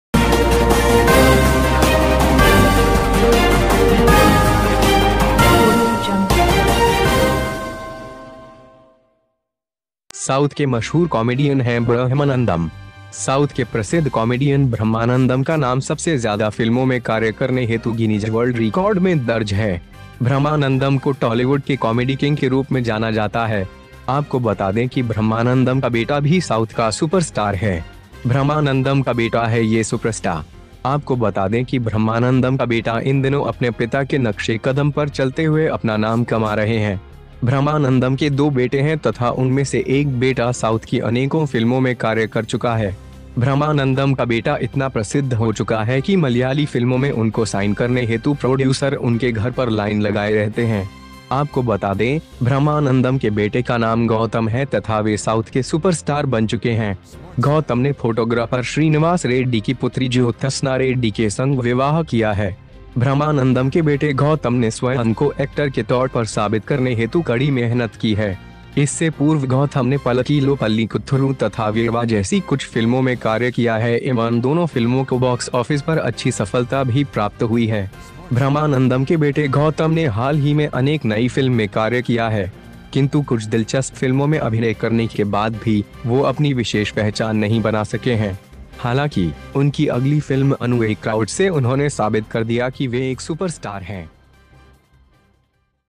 इस कॉमेडियन का 'बेटा' भी है टॉलीवूड का सुपरस्टार, देखें कैसा दिखता है वो
Name of the famous comedian Brahmanandam of South is recorded in 'Guinness World Records' for most films. Brahmanandam is known as Tollywood's Comedy King. Let us tell you that Brahmanandam's son is also South's superstar.
It is the son of Brahmanandam
Let us tell you that the son of Brahmanandam is earning his name while walking on the steps of his father these days. Brahmanandam has two sons and one of them has worked in many South South films
The son of Brahmanandam has become so popular that in order to sign him in Malayalam films, the producer keeps on line at his house. Let us tell you that the name of Brahmanandam's son is Gautama and he has become the superstar of South
Gautam has married photographer Srinivas Reddy's daughter Jyothsna Reddy. Gautam, son of Brahmanandam, has worked hard to prove himself as an actor.